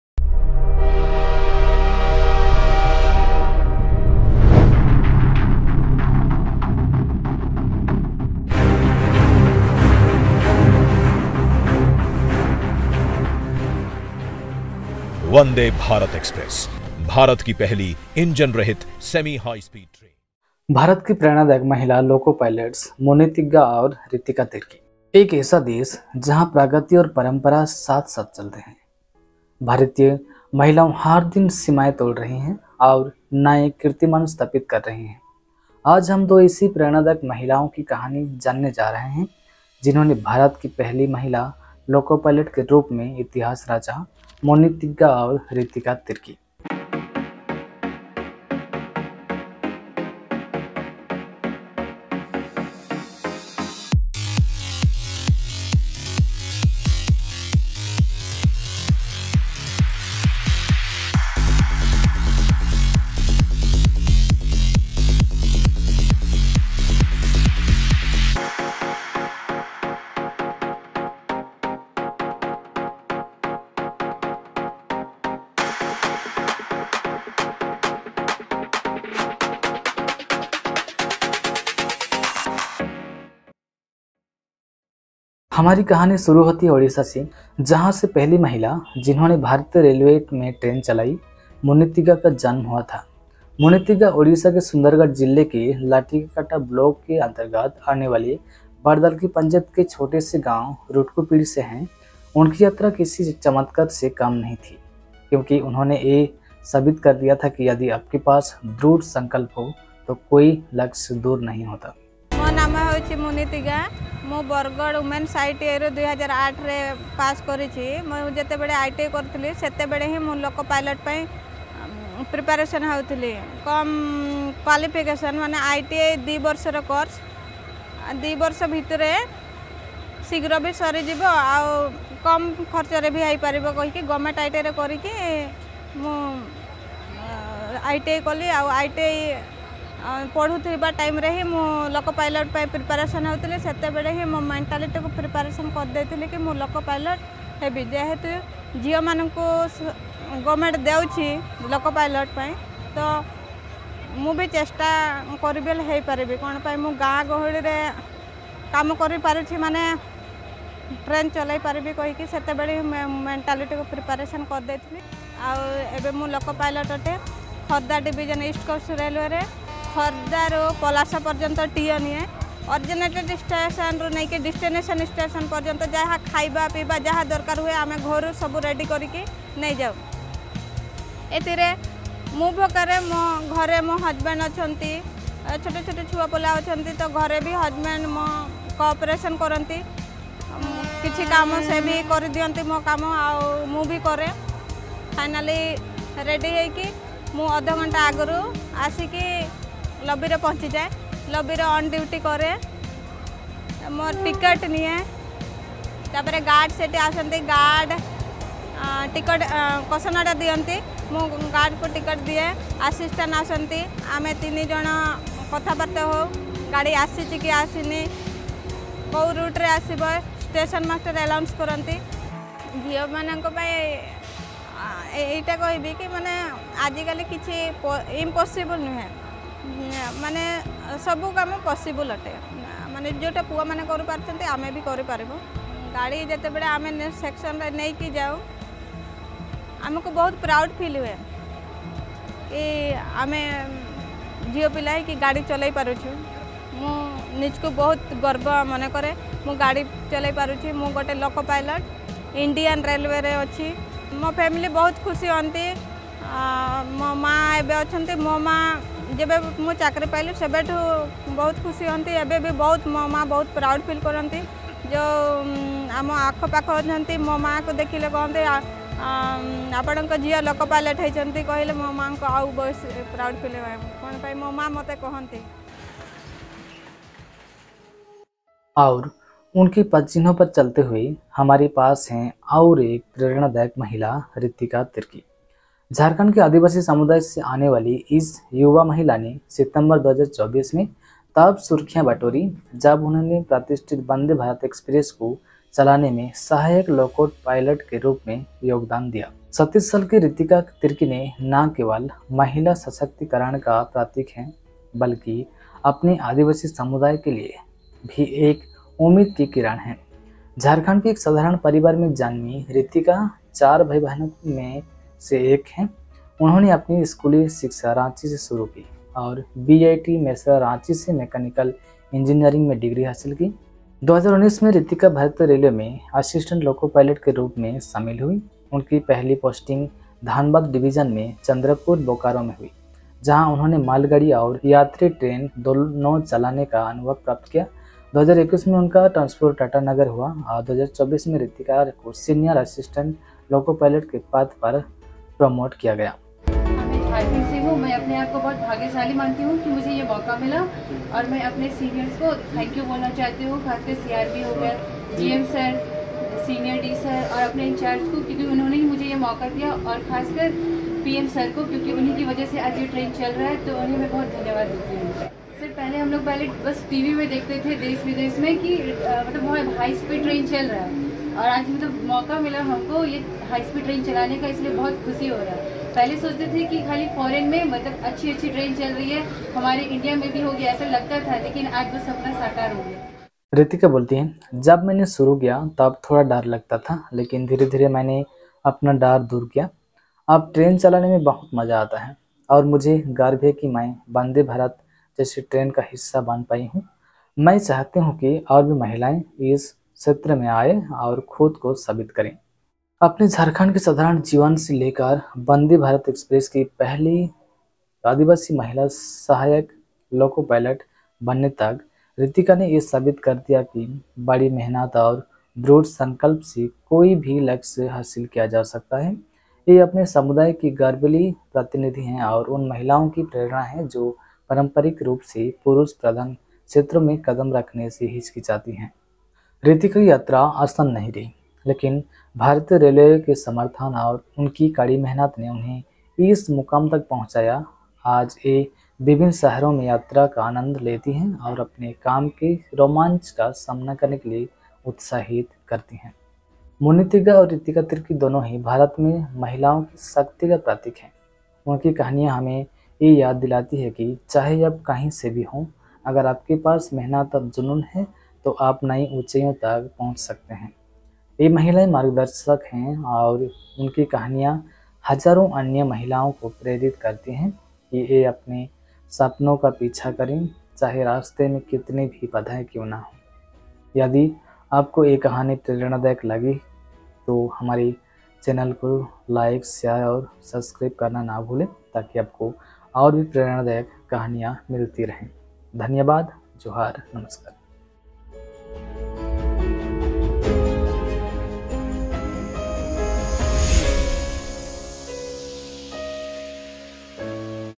Muni Tigga & Ritika Tirkey: First Tribal Loco Pilots of Vande Bharat Express | Historic Journey
In this inspiring video, we celebrate the incredible achievement of Muni Tigga and Ritika Tirkey, who have made history as the first tribal loco pilots of the prestigious Vande Bharat Express. From humble beginnings to reaching new heights, their journey is a powerful testament to hard work, determination, and breaking barriers.

Learn about their personal stories, the challenges they overcame, and how they became pioneers in the world of Indian Railways. Their success not only brings pride to their tribal communities but also serves as an inspiration for millions of people across India.

Watch this video to discover how Muni Tigga and Ritika Tirkey are paving the way for future generations, proving that no dream is too big when you have the courage to pursue it.

My used Gear :-
Redmi Note 11 Pro Plus 5g
Canon EOS 200D
Boya ByM1 Mic
Interview Mic V-Audio Pro Reporter mic
Green Screen